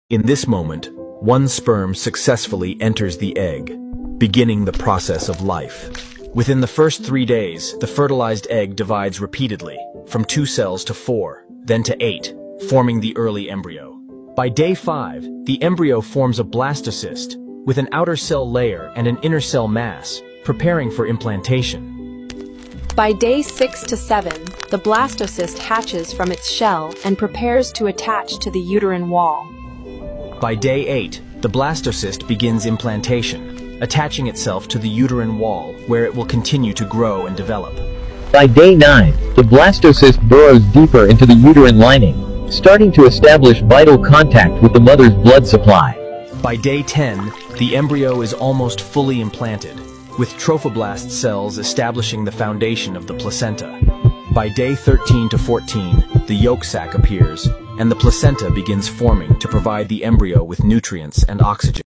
Fetal Development
✨ The Miracle of Fetal Development ✨
From a single fertilized cell to a fully formed baby — the journey of life is truly extraordinary. Throughout the weeks of pregnancy, the fetus grows and transforms: the heart begins to beat, tiny limbs appear, and the brain develops rapidly. Each stage marks a beautiful step in the creation of new life. 👶💫
Experience the incredible process of human development inside the womb — where science meets the miracle of life.